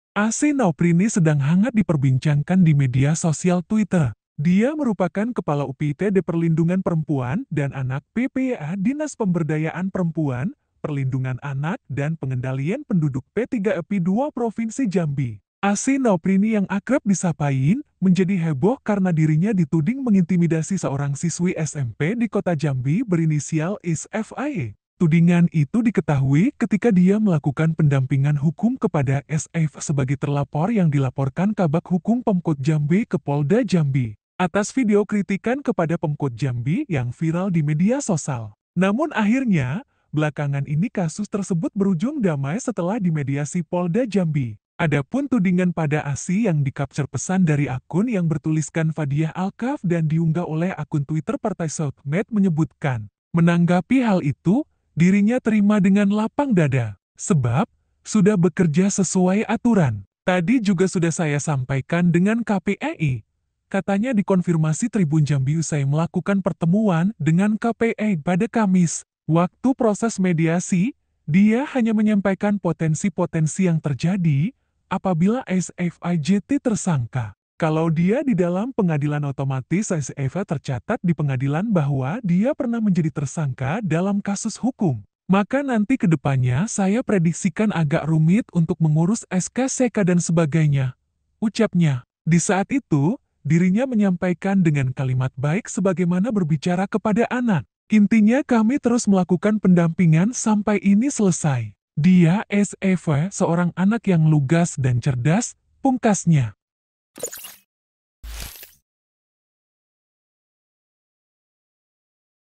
KLARIFIKASI ASI NOPRINI Diduga Intimidasi SFA Saat Mediasi di Polda Jambi: Tidak Maksud Intimidasi
VP: Erric

Dia merupakan Kepala UPTD Perlindungan Perempuan dan Anak (PPA) Dinas Pemberdayaan Perempuan, Perlindungan Anak dan Pengendalian Penduduk (P3AP2) Provinsi Jambi. Asi Noprini yang akrab disapa Iin, menjadi heboh karena dirinya dituding mengintimidasi seorang siswi SMP di Kota Jambi berinisial SFA. Tudingan itu diketahui ketika dia melakukan pendampingan hukum kepada SFA sebagai terlapor yang dilaporkan Kabag Hukum Pemkot Jambi ke Polda Jambi.

Atas video kritikan kepada Pemkot Jambi yang viral di media sosal. Namun akhirnya, belakangan ini kasus tersebut berujung damai setelah dimediasi Polda Jambi. Adapun tudingan pada Asi yang dicapture pesan dari akun yang bertuliskan Fadiyah Alkaff dan diunggah oleh akun Twitter PartaiSocmed menyebutkan. Menanggapi hal itu, dirinya terima dengan lapang dada. Sebab, sudah bekerja sesuai aturan.

“Tadi juga sudah saya sampaikan dengan KPAI,” katanya dikonfirmasi Tribunjambi usai melakukan pertemuan dengan KPAI pada Kamis (8/6/2023). Waktu proses mediasi, dia hanya menyampaikan potensi-potensi yang terjadi apabila SFA jadi tersangka.

“Kalau dia di dalam pengadilan otomatis SFA tercatat di pengadilan bahwa dia pernah menjadi tersangka dalam kasus hukum. Maka nanti ke depannya saya prediksikan agak rumit untuk mengurus SKCK dan sebagainya,” ucapnya.

Di saat itu, dirinya menyampaikan dengan kalimat baik sebagaimana berbicara kepada anak. “Intinya kami terus melakukan pendampingan sampai ini selesai. Dia (SFA) seorang anak yang lugas dan cerdas,” pungkasnya.